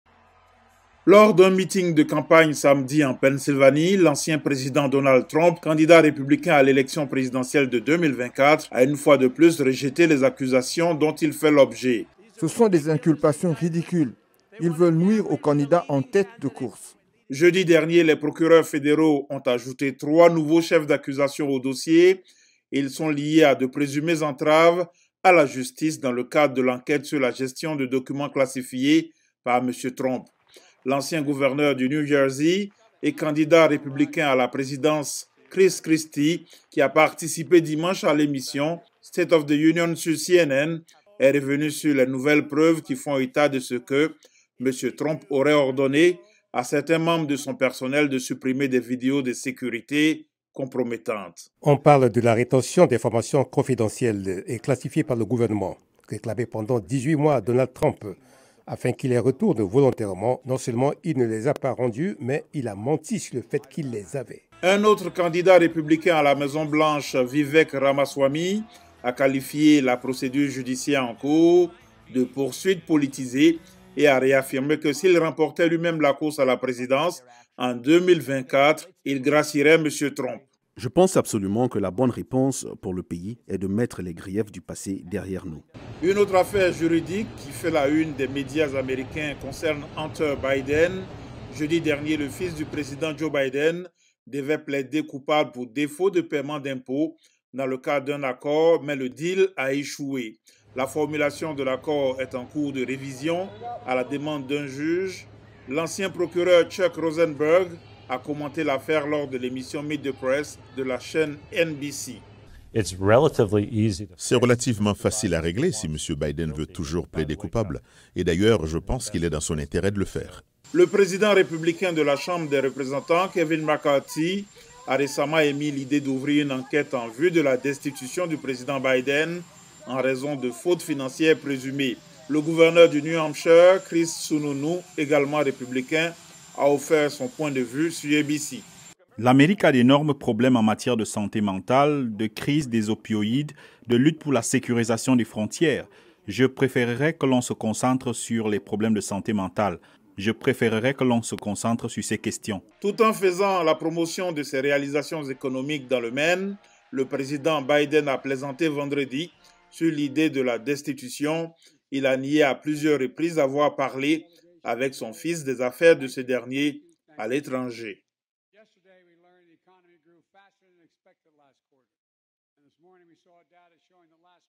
Donald Trump et Hunter Biden confrontés à des ennuis judiciaires
Donald Trump a déclaré qu'une condamnation ne lui ferait pas arrêter sa campagne présidentielle. Parallèlement, Hunter Biden est confronté à des ennuis judiciaires qui placent son père, le président démocrate Joe Biden, dans la ligne de mire des républicains.